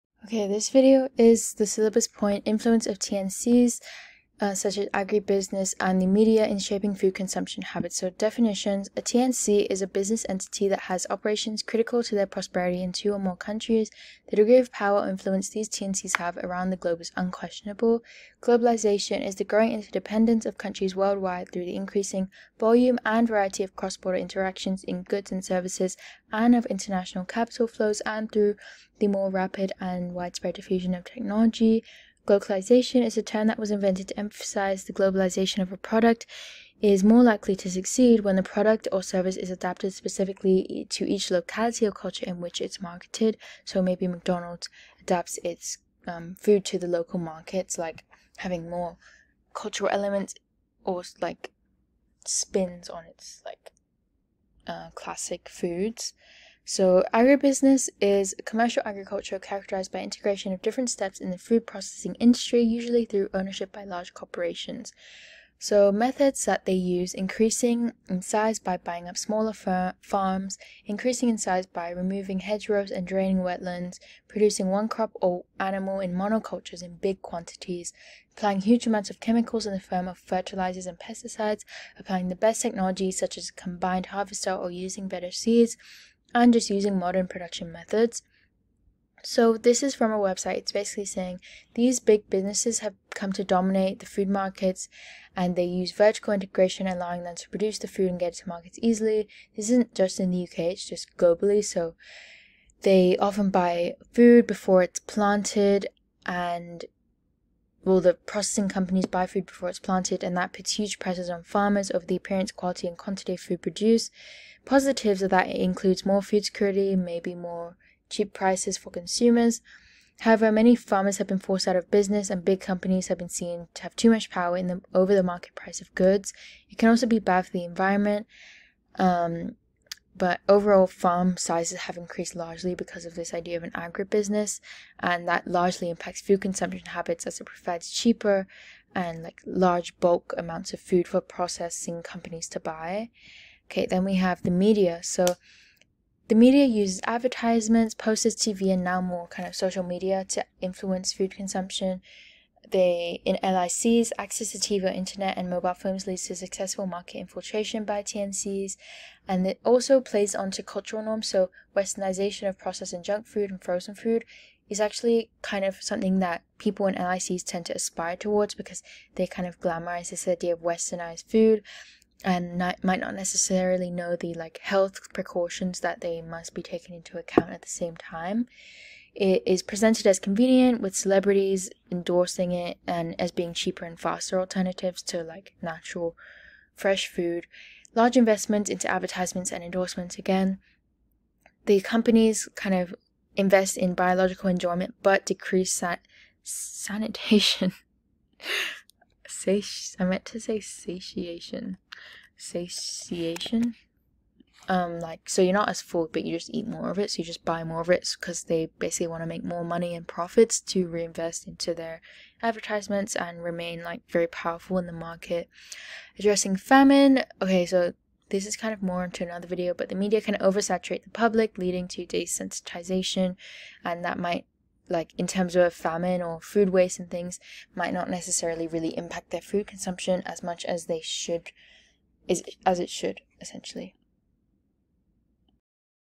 IB Geography: The influence of TNCs (agribusinesses and the media) in shaping food consumption
IB Geography
Food and Health
The influence of TNCs (agribusinesses and the media) in shaping food consumption
habits